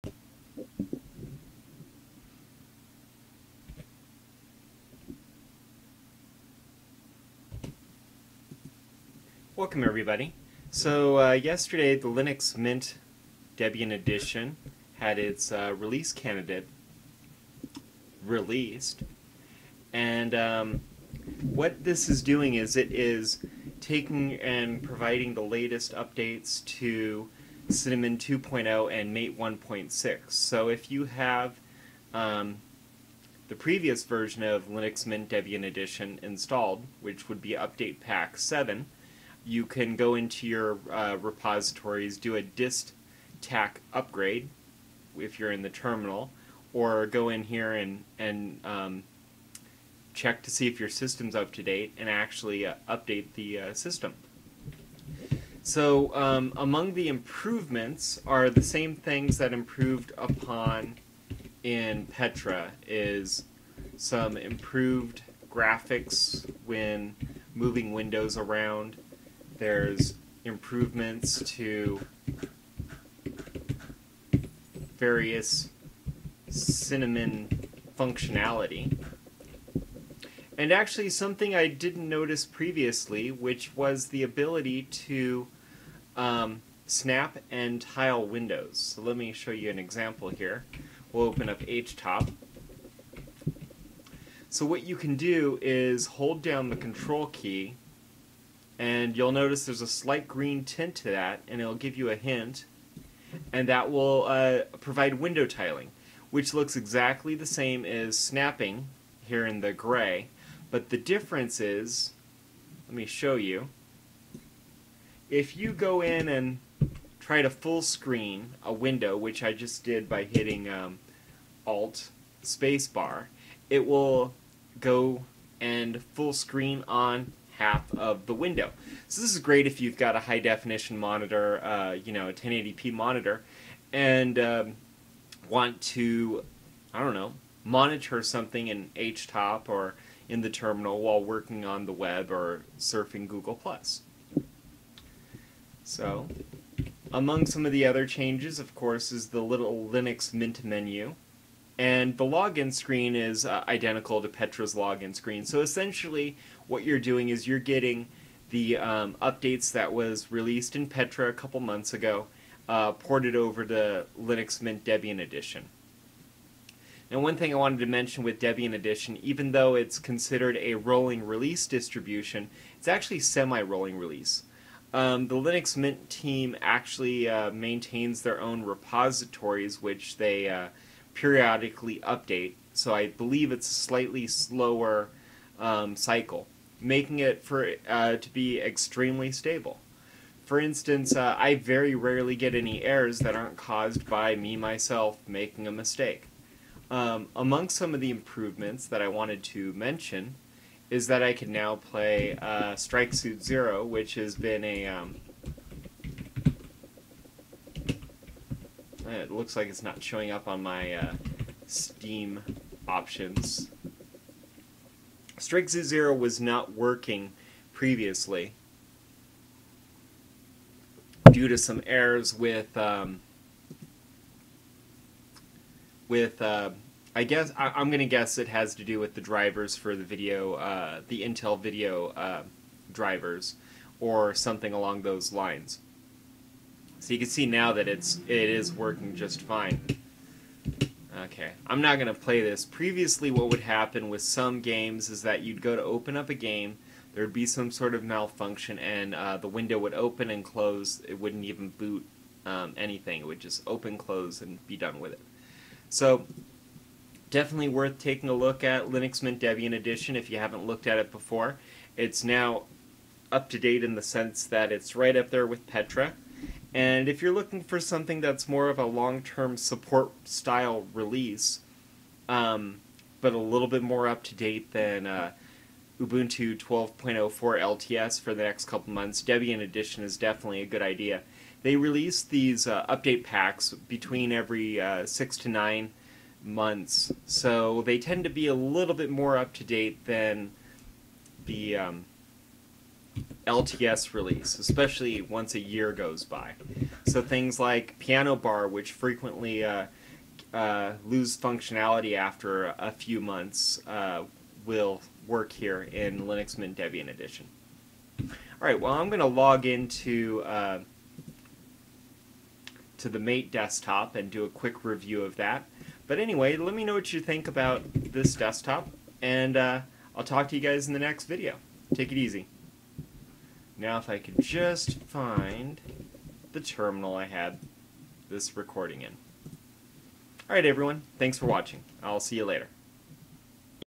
Linux Mint Debian Edition Update Pack 8 - Cinnamon
Linux Mint Debian Edition Update Pack 8's release candidate just arrived. Here is a quick overview of the new features brought to the Cinnamon desktop.